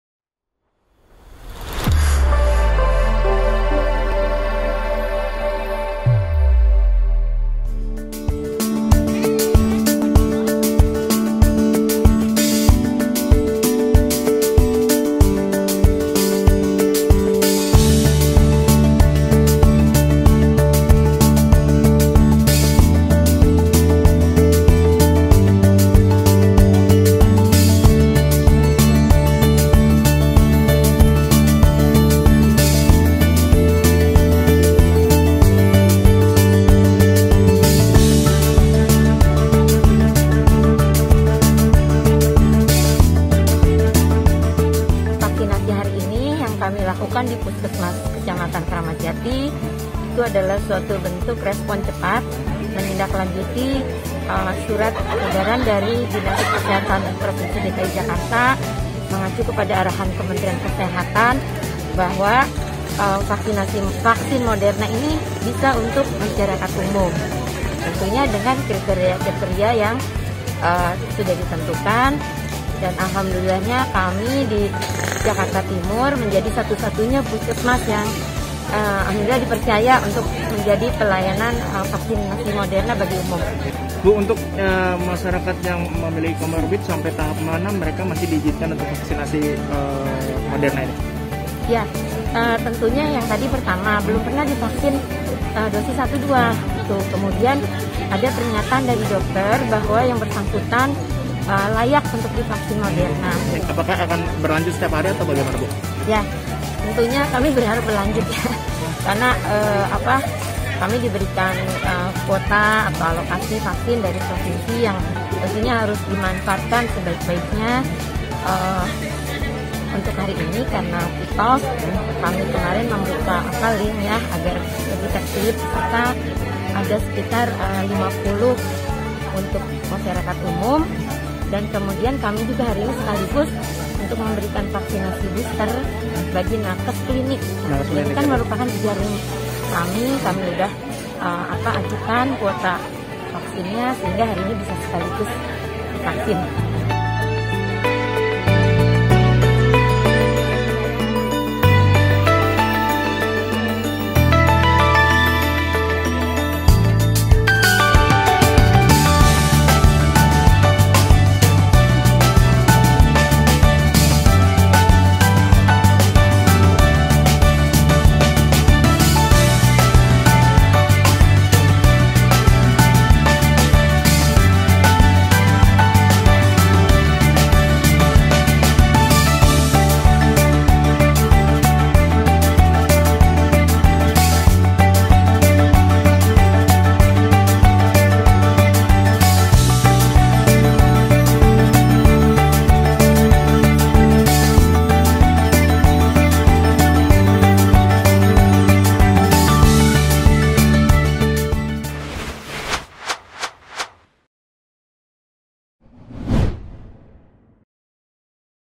PUSKESMAS KRAMAT JATI LAKUKAN PENYUNTIKAN VAKSIN COVID-19 MODERNA BAGI NAKES DAN MASYARAKAT UMUM
JAKARTA (Poskota) - Suku Dinas Kesehatan Jakarta Timur mulai lakukan penyuntikan dosis vaksinasi Covid-19 berjenis Vaksin Moderna untuk tenaga kesehatan (nakes) dan masyarakat umum.

Vaksinasi Covid-19 jenis Moderna pertama kali di Jakarta Timur, tepatnya di Puskesmas Kecamatan Kramat Jati.

Kepala Puskesmas Kecamatan Kramat Jati, Indah Mutiara menyampaikan bahwa kantornya jadi tempat vaksinasi Covid-19 booster satu-satunya di Jakarta Timur